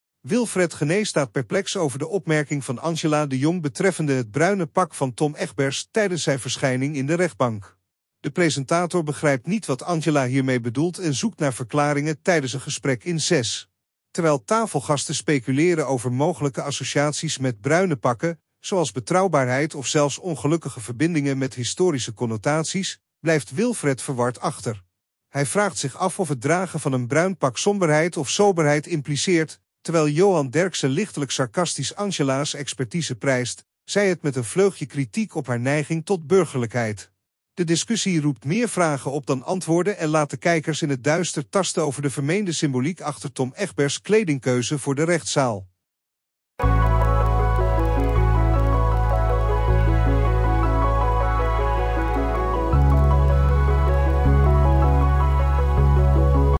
Wat bedoelt Tom Egbers met dat bruine pak? Wilfred Genee snapt er niks van!
Wilfred Genee en tafelgasten speculeren over Angela de Jong's opmerking over Tom Egbers' bruine pak in de rechtbank. Verwarring en discussie volgen.